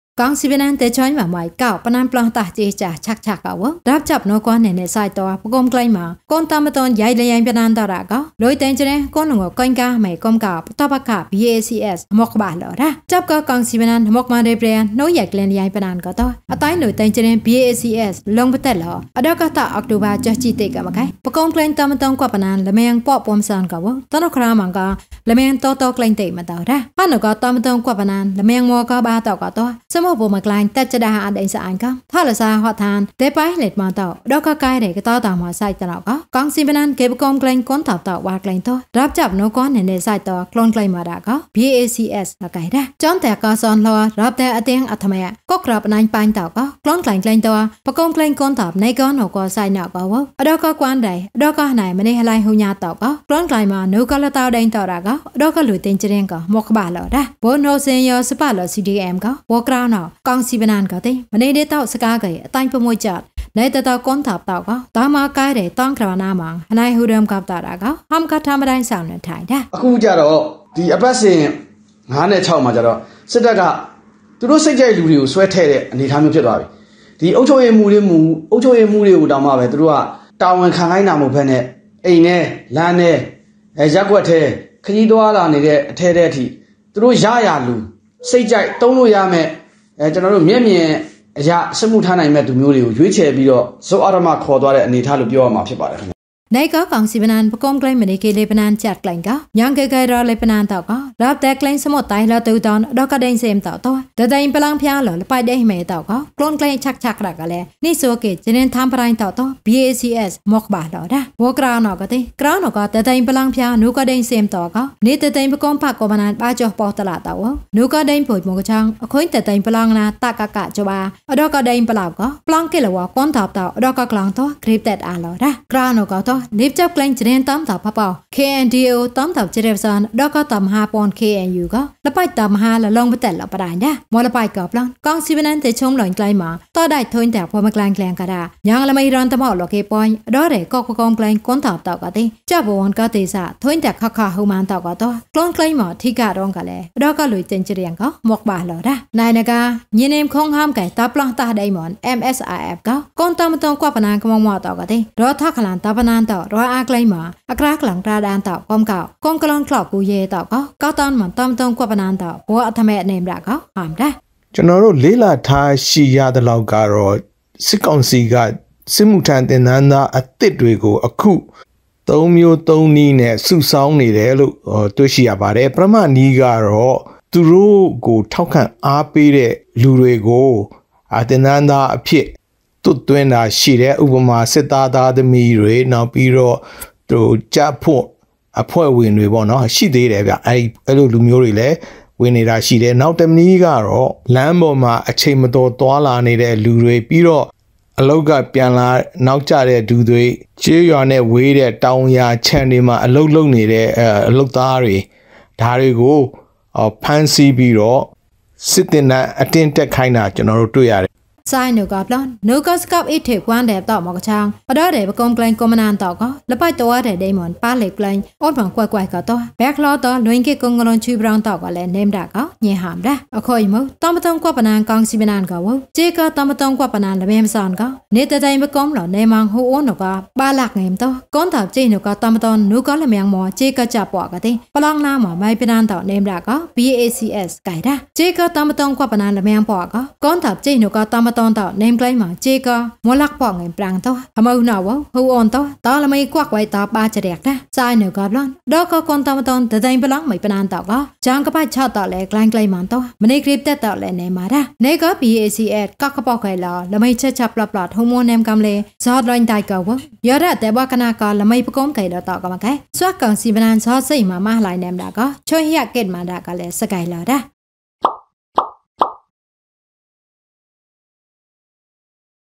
TLT Tv || - ရပ်စပ်ကၠုၚ်နဲကဲနာနာတုဲ ကံၚ်ဇြဳပၞာန် ပကောံကၠုၚ်ကောန်ဒပ်
ကံၚ်ဇြဳပၞာန် ဒးဆေၚ်ဒၟံၚ်မုက် ကုပၞာန်ပၠန်ဂတးဒစဵုဒစး အဆက်က်ဂှ်ဝွံ ရပ်စပ်နဲကဲနာနာသာ်တုဲ ပကောံကၠုၚ်ဒၟံၚ် ကောန်တန်ဗ္တောန်ယိုက်လျိုၚ်ပၞာန်တံဂှ် လိက်ဒုၚ်စဳရေၚ် ဂကောံလ္ၚတ်ကေၚ်ကာဗမာ ကေုာံ ပဋိပက္ခ BACS ထ္မံက်ထ္ၜးလဝ်ရ။